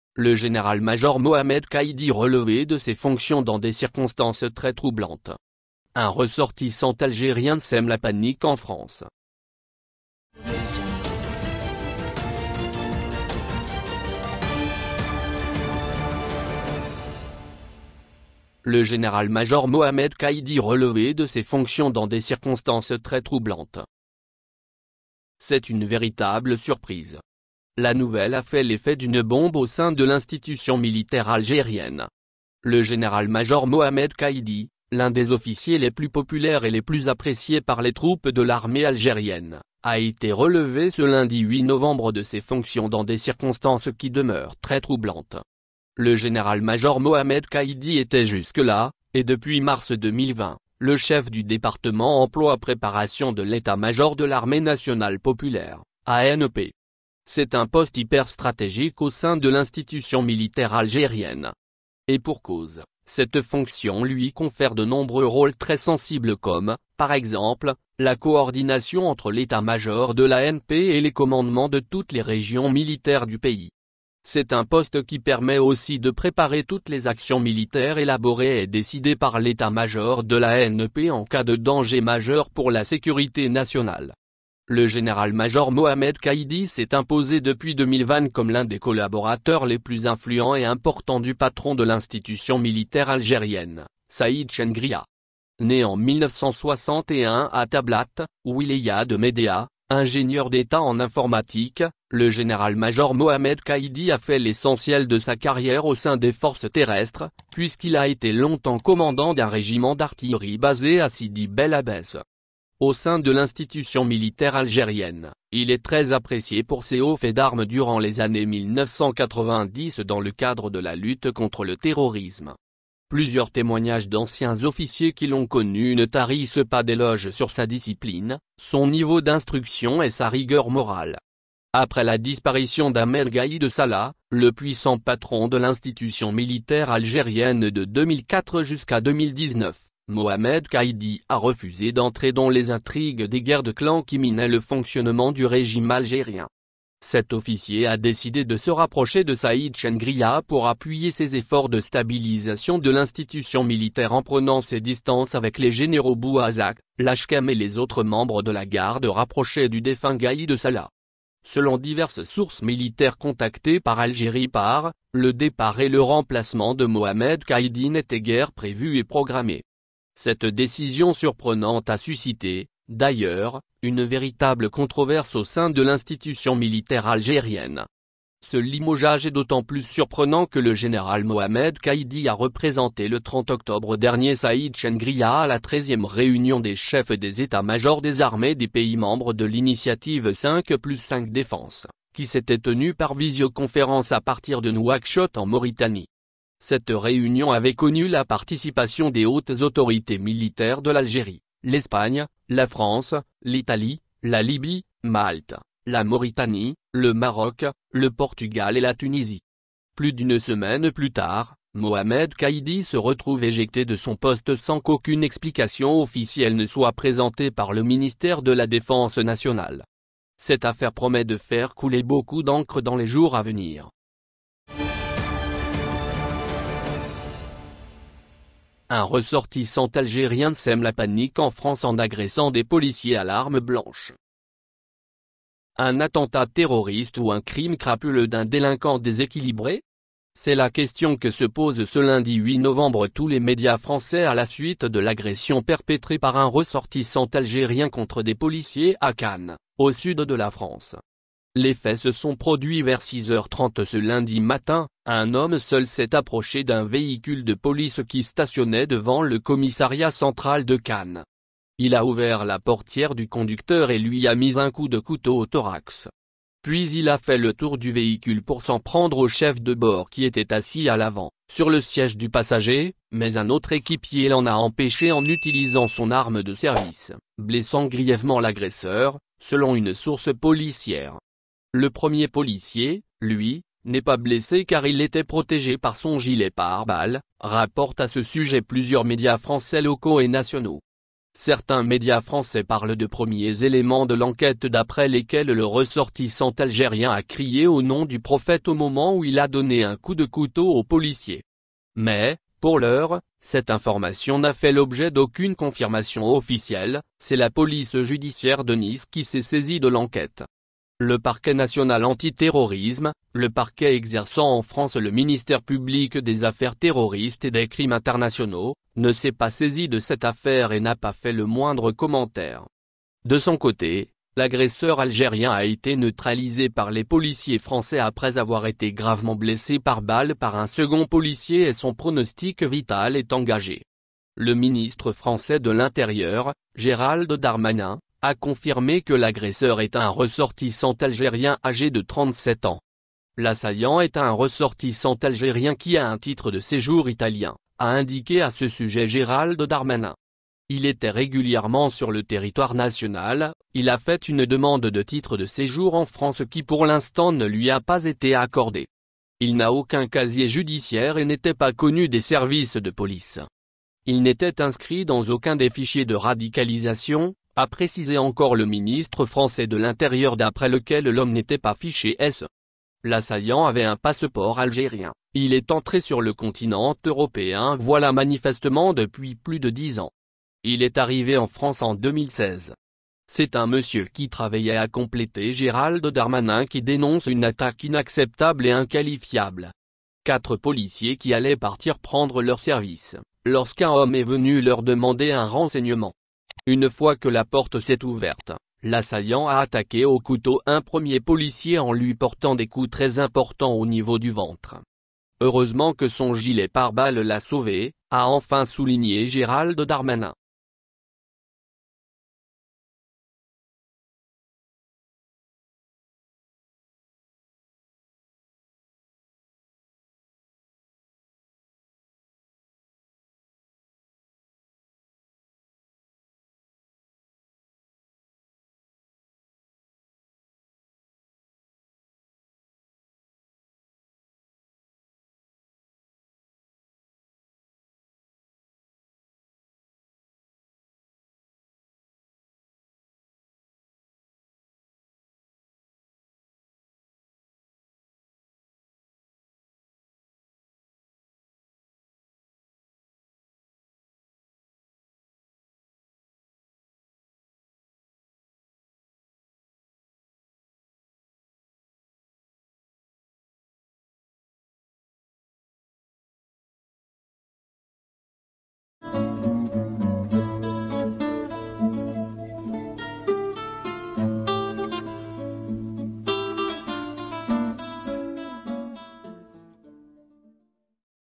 Le Général Mohamed Kaidi relevé de ses fonctions | Un algérien sème la panique en France...
C’est une véritable surprise. La nouvelle a fait l’effet d’une bombe au sein de l’institution militaire algérienne. Le Général-Major Mohamed Kaidi, l’un des officiers les plus populaires et les plus appréciés par les troupes de l’armée algérienne, a été relevé ce lundi 8 novembre de ses fonctions dans des circonstances qui demeurent très troublantes.
Un attentat terroriste ou un crime crapuleux d’un délinquant déséquilibré ? C’est la question que se pose ce lundi 8 novembre tous les médias français à la suite de l’agression perpétrée par un ressortissant algérien contre des policiers à Cannes au sud de la France.